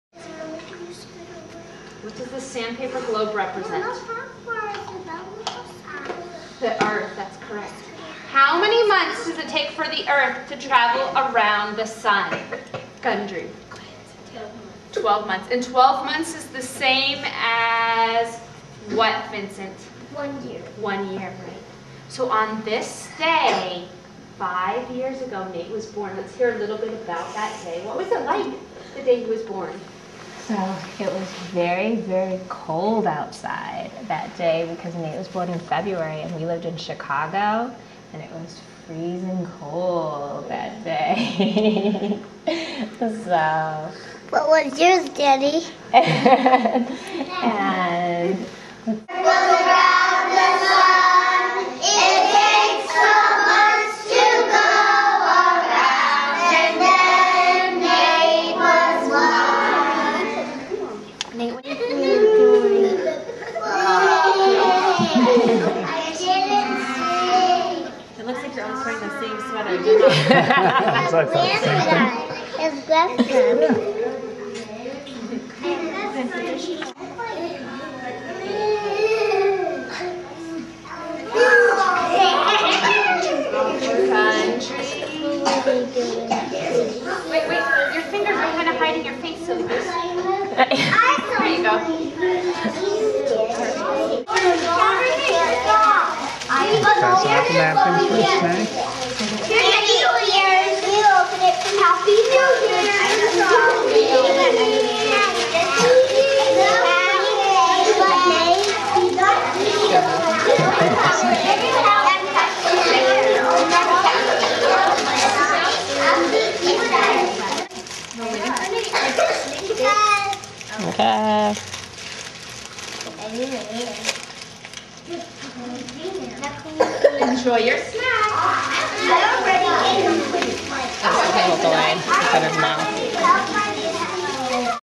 Nate's Birthday at School
Nate celebrates his 5th birthday at school